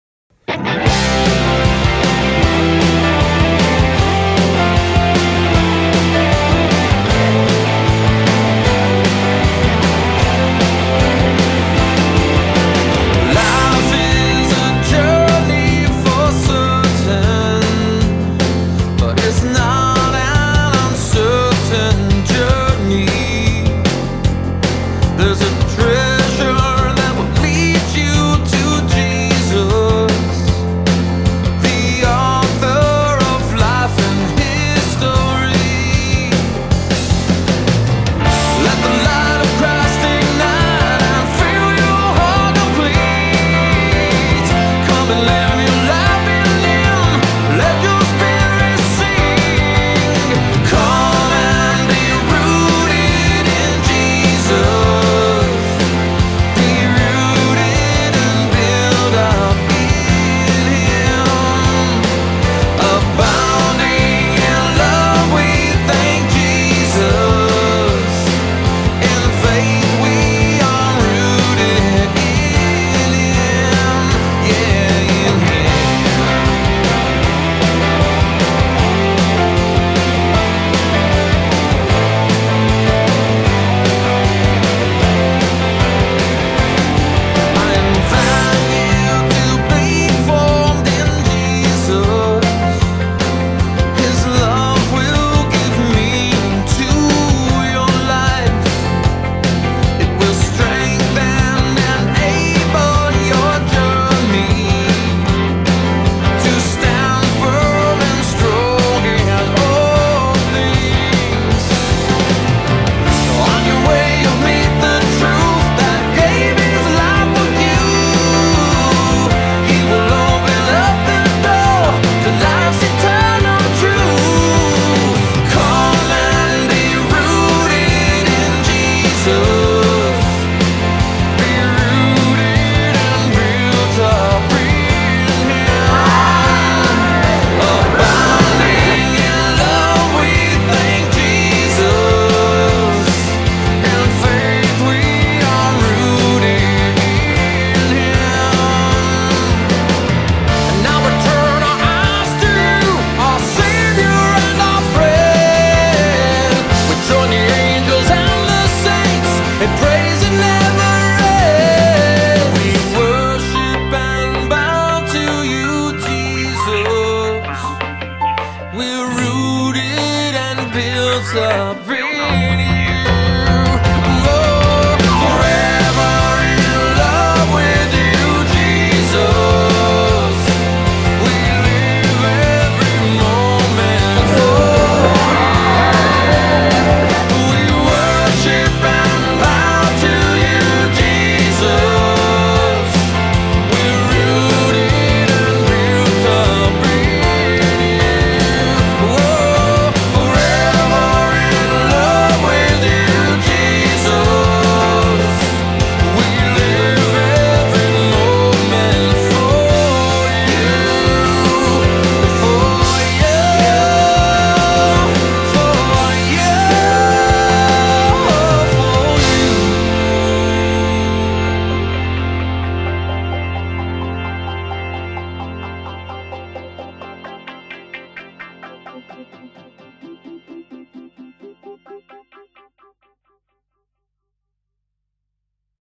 San Clemente "Rooted" Steubenville of the Rockies 2011, Day 2
The video is highlights of day one of San Clemente's Life Teen group at the Steubenville of the Rockies Catholic youth conference.